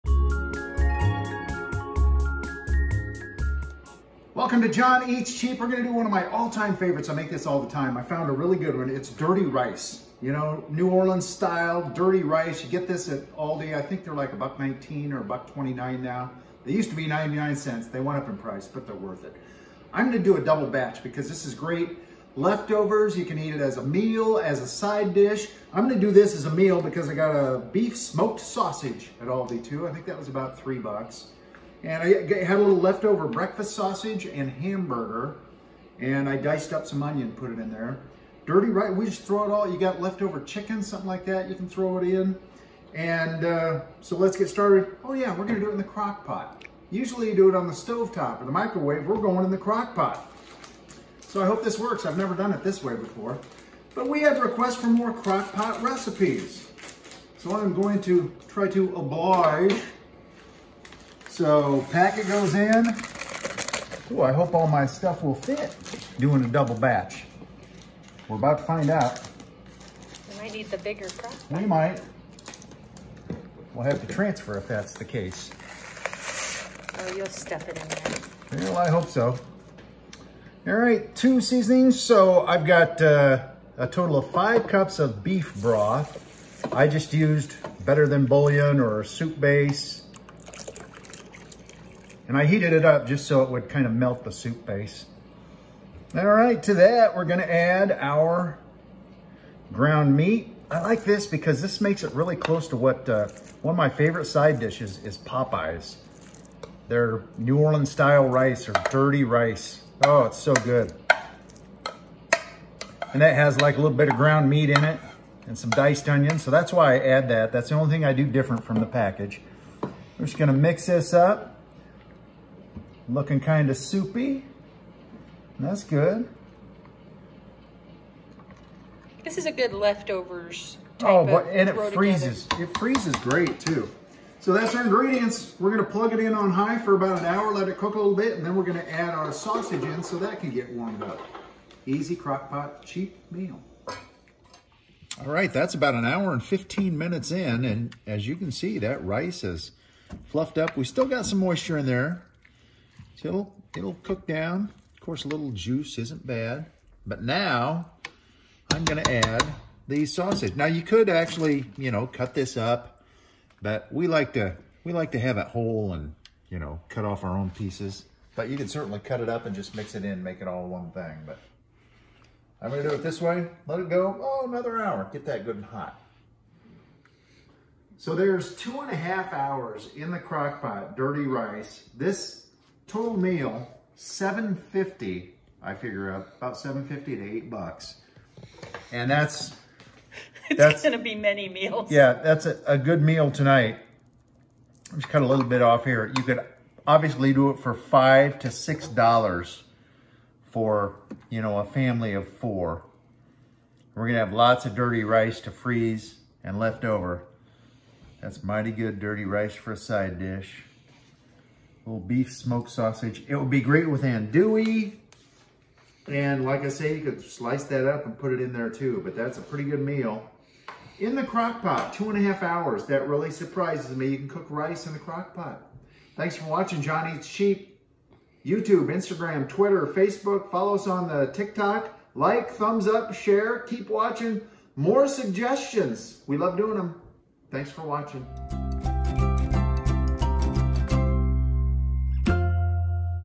Crockpot Dirty Rice | Easy Slow Cooker Rice | John Eats Chep
John loves Popeye’s dirty rice side dish and recreated it using Aldi dirty rice and leftovers. Done in a couple hours, it makes a hearty, easy weeknight meal.